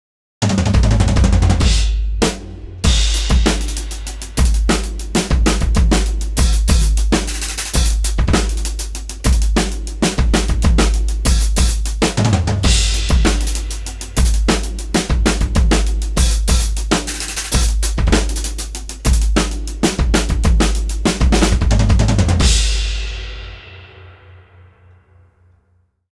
Fun With Roland Kick & Snare Triggers
Running triggers through a Roland SPD-SX. Yamaha Absolute hybrid shells and Zildjian Special Dry pies.